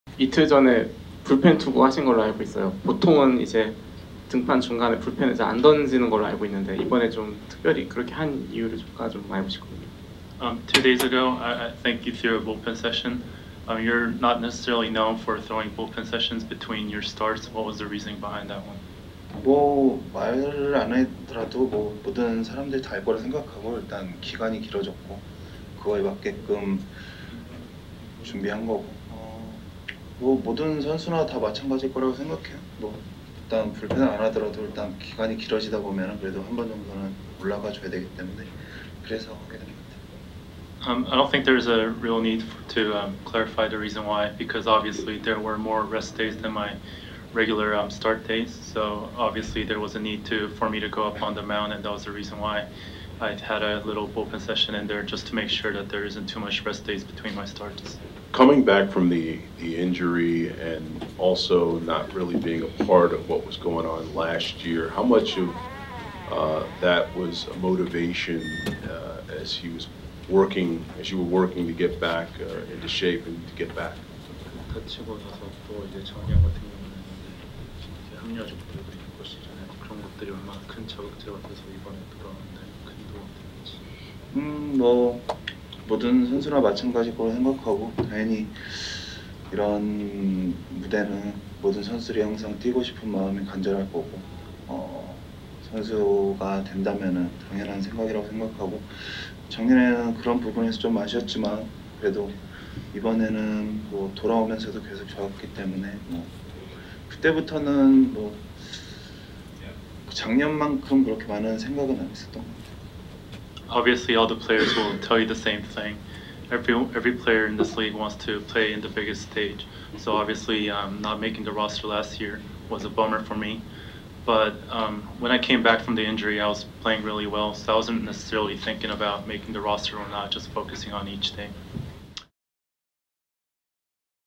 Dodgers NLCS 2018: Hyun-jin Ryu on starting Game 2 of the NLCS
Los Angeles Dodgers pitcher Hyun-jin Ryu talks about starting Game 2 of the NLCS against the Milwaukee Brewers.